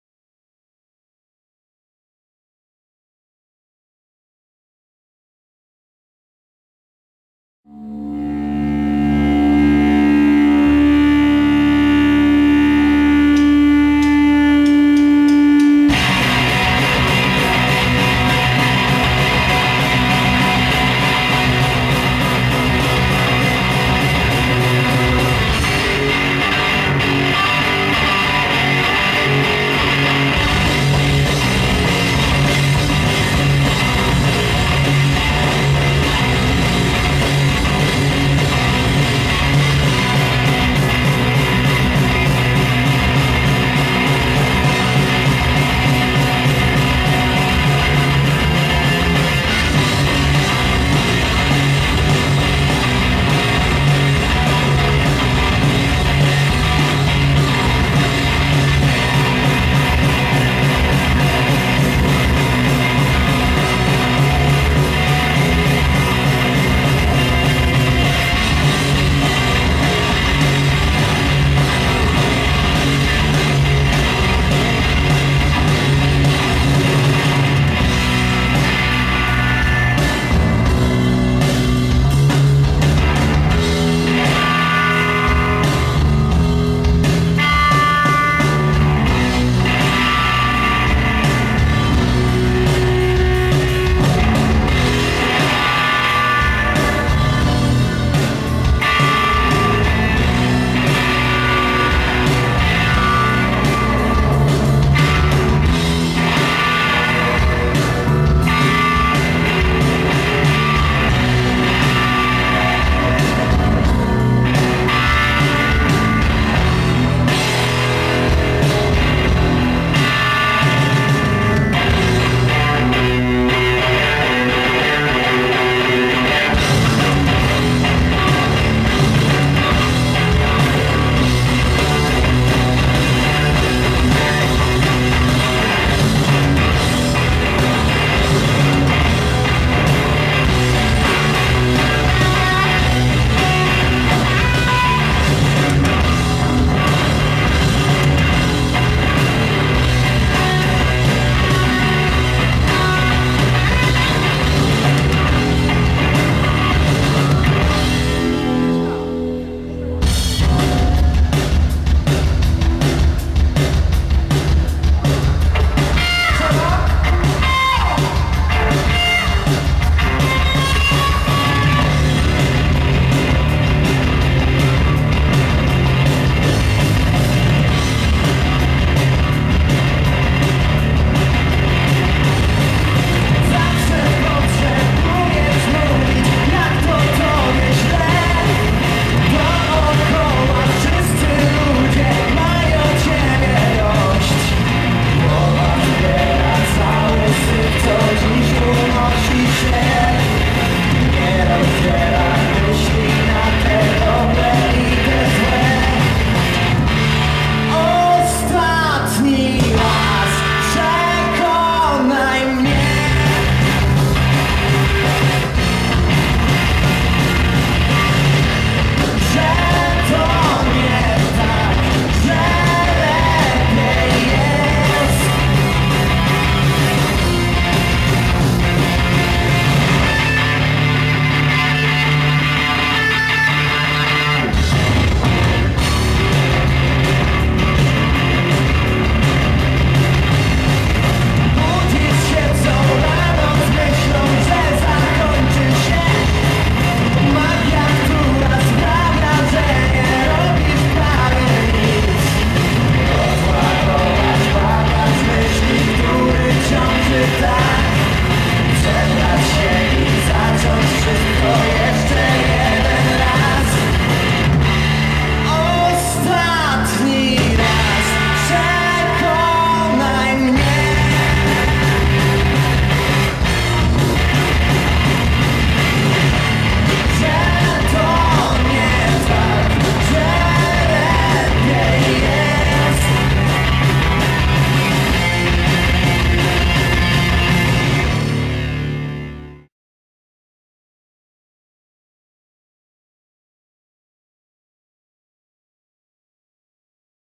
Zde(a)rzenia 2012: Sayes (30.06.2012 r., Tarnów)
Fragment koncertu zespołu Sayes w ramach Zde(a)rzeń 2012. Występ miał miejsce 30 czerwca 2012 roku na tarnowskim Rynku.